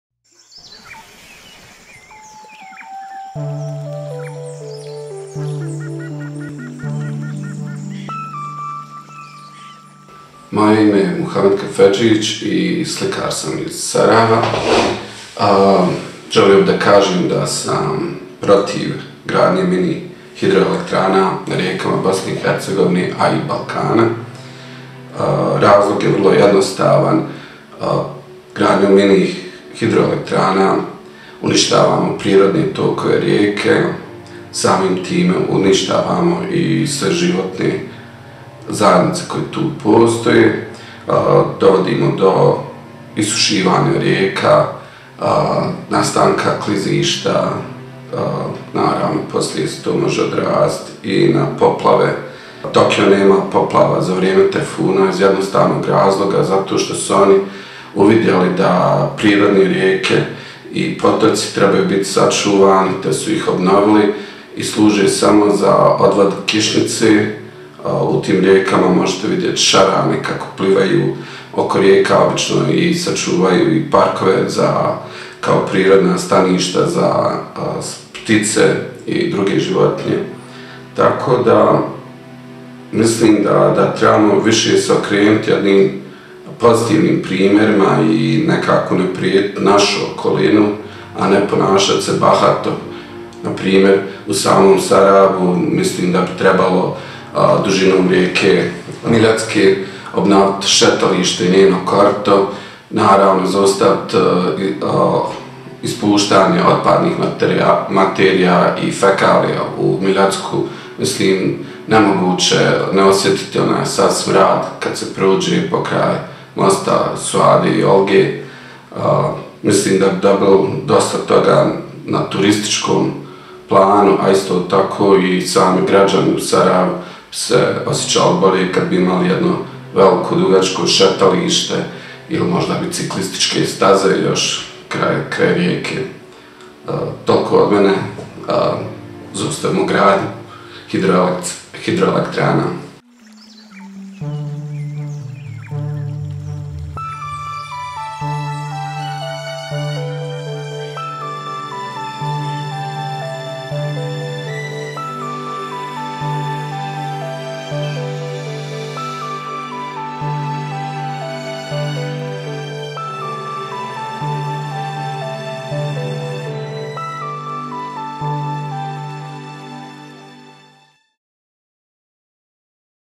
Artists speak up for Balkan Rivers / Umjetnici za rijeke/Artists for Rivers
++ On the occasion of the World Music Day on June 21st, 40 artists support the Blue Heart campaign for the protection of Balkan Rivers ++
I am one of them.